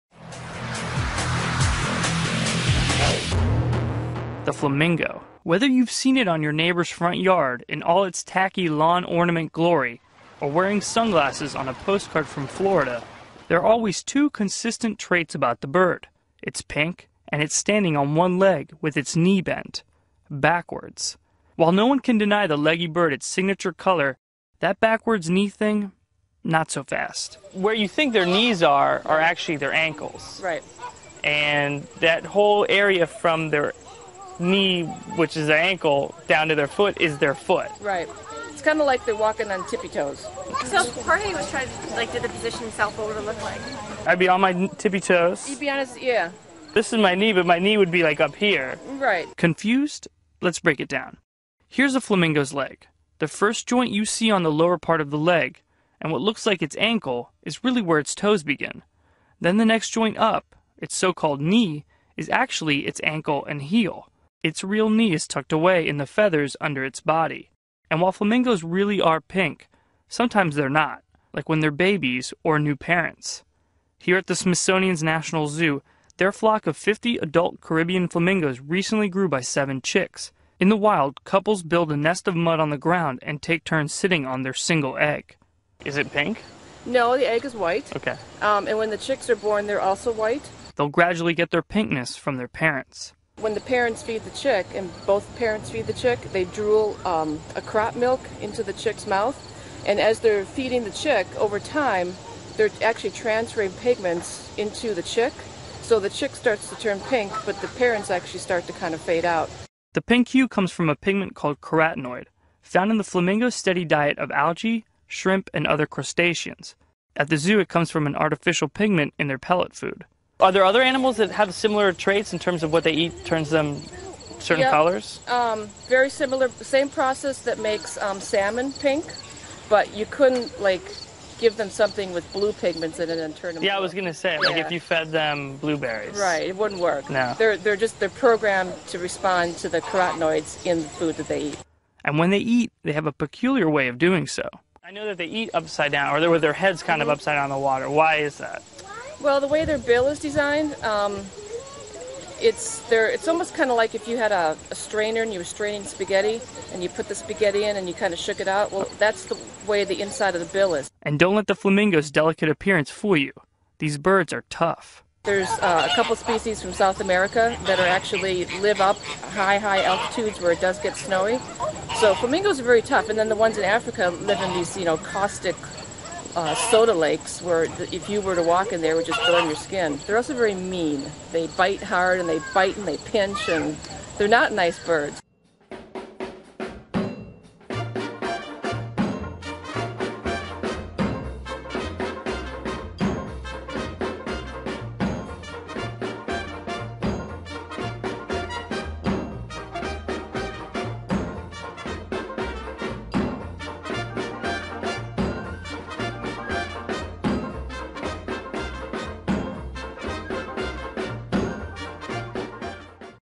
Flamingos!
Facts about flamingos:
Why it's Pink?
Why it's knees bent backwards?
Why they ate upside down?